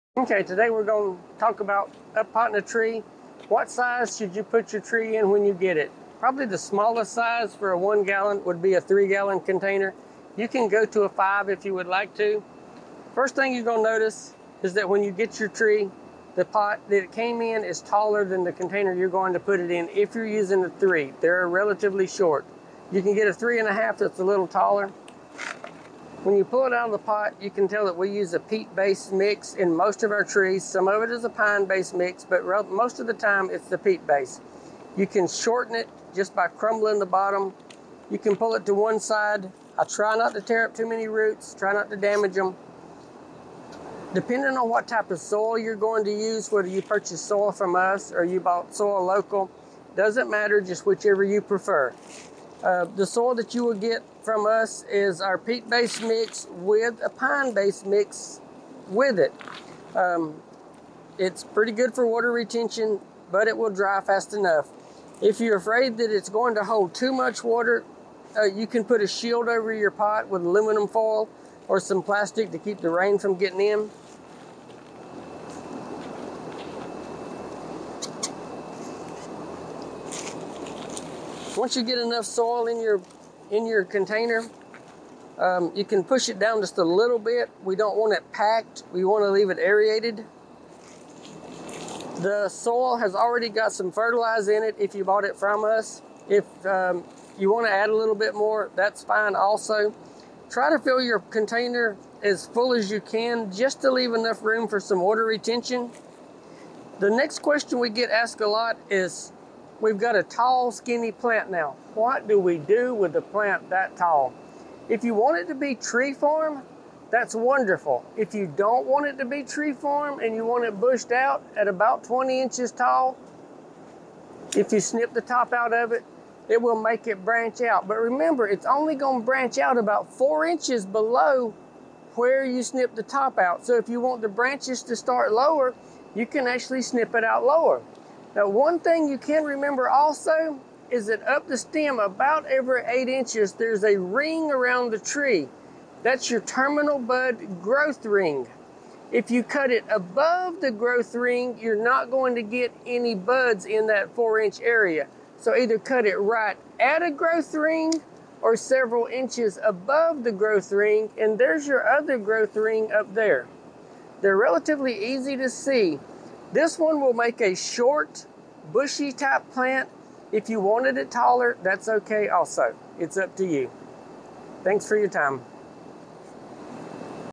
How To Uppot Your New Citrus Tree - Madison Citrus Nursery
We have had a few customers asking about uppotting their new citrus tree so we decided to make a video showing how we recommend doing it. We hope this helps with the care of your new potted citrus.